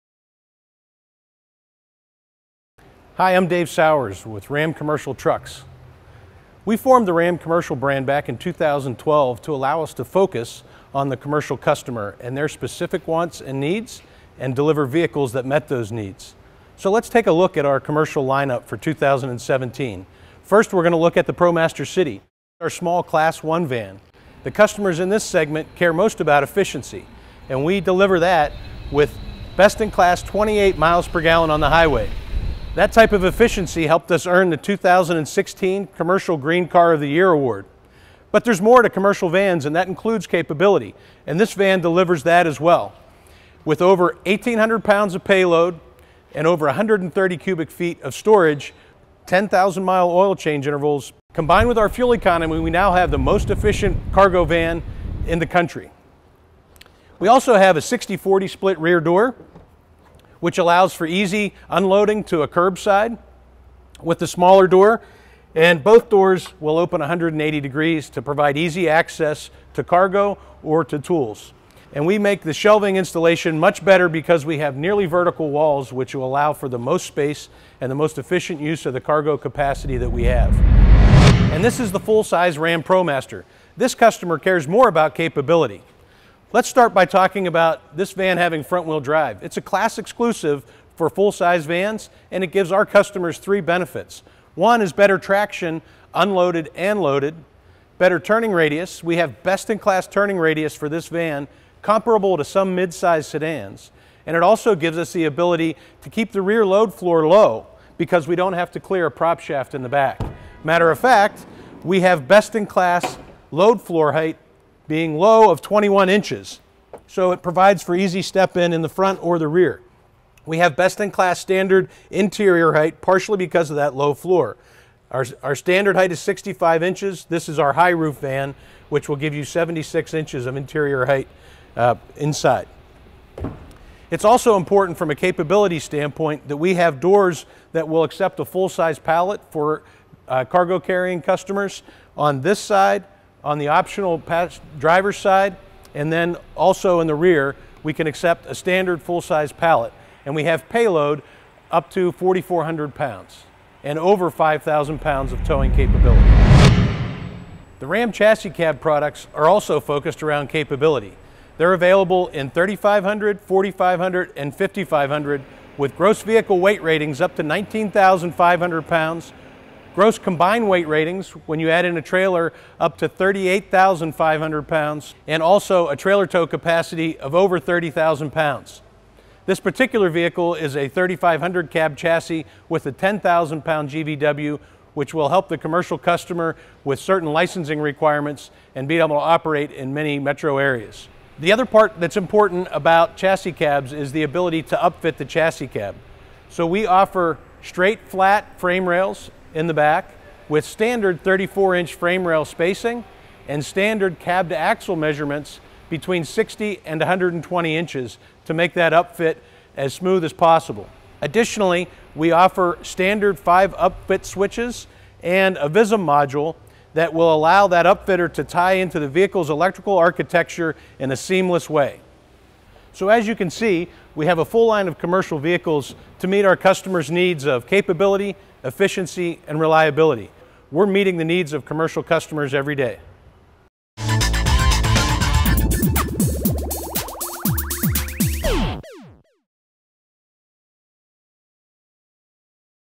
Ram Commercial Trucks
Ram Truck builds America's longest-lasting pickups, and that includes a full line-up of workhorses to meet the needs and capability demanded by commercial customers. Dave Sowers, Head of Ram Commercial Marketing, takes a closer look at the features and capability commercial customers will find in the 2017 Ram ProMaster, ProMaster City, and Ram 3500, 4500 and 5500 Chassis CabTrucks.